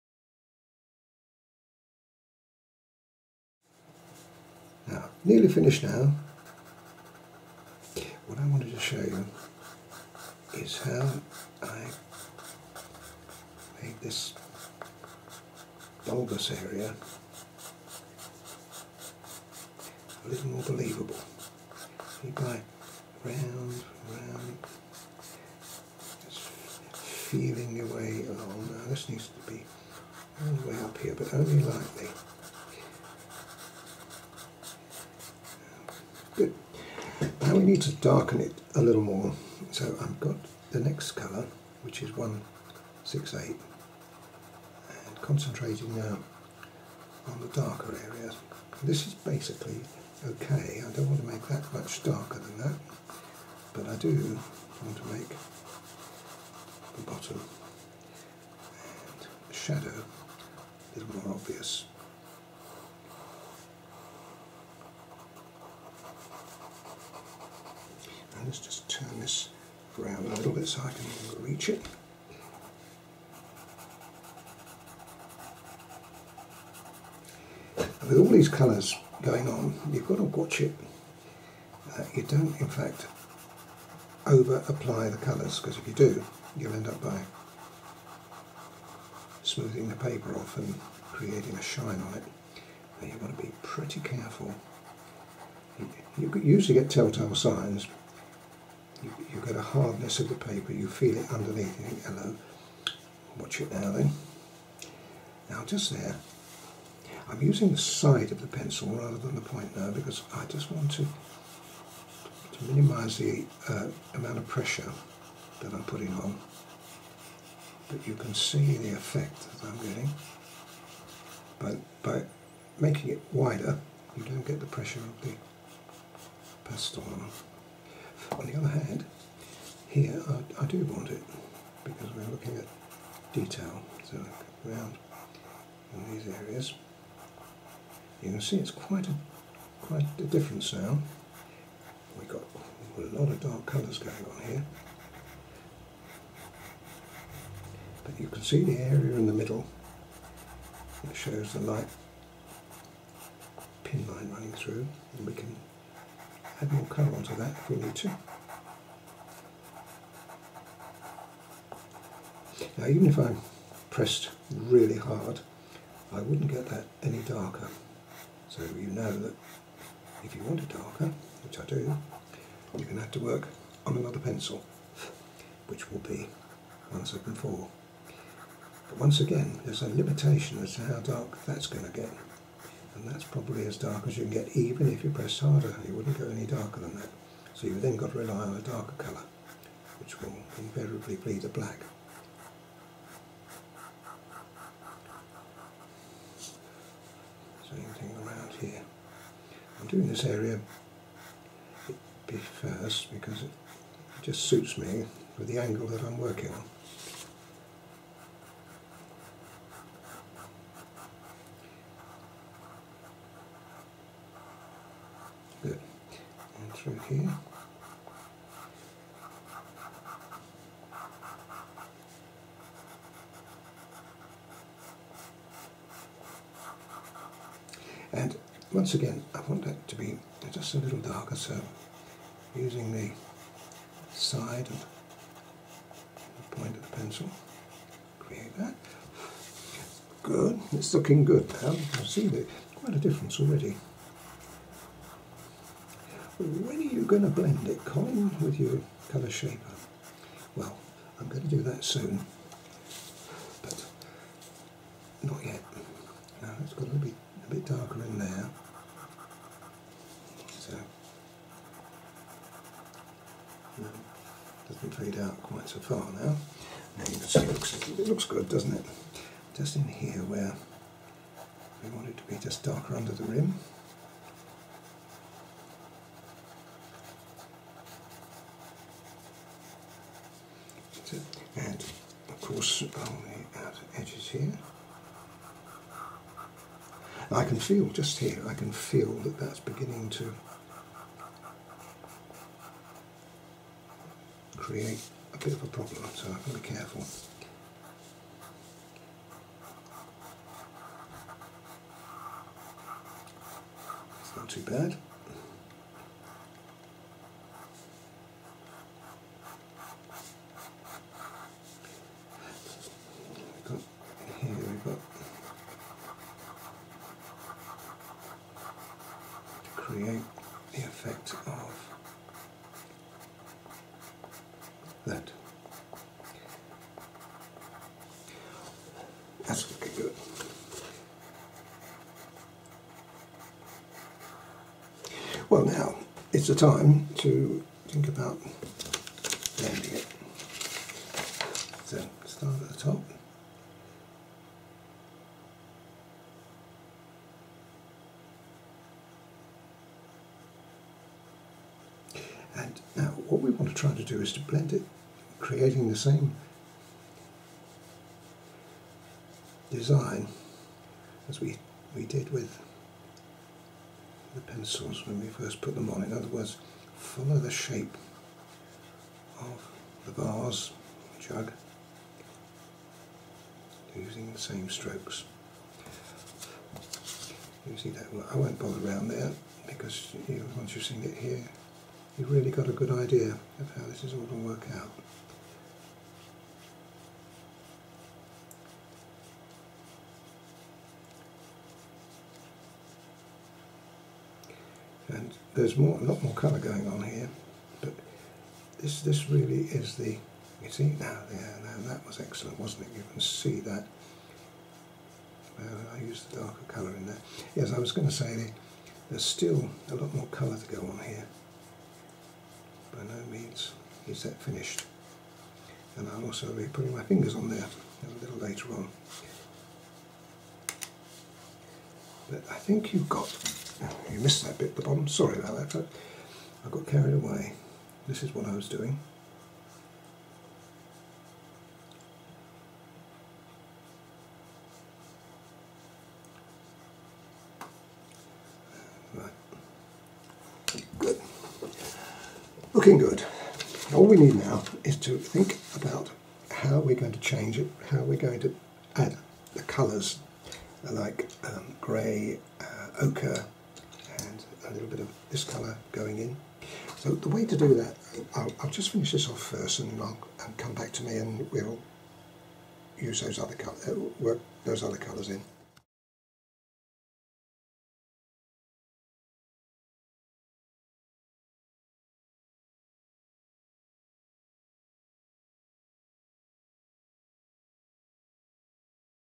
Vase of Roses - Vase Tutorial
In this short demo, Colin demonstrates how to use pastel pencils for the vase for the roses.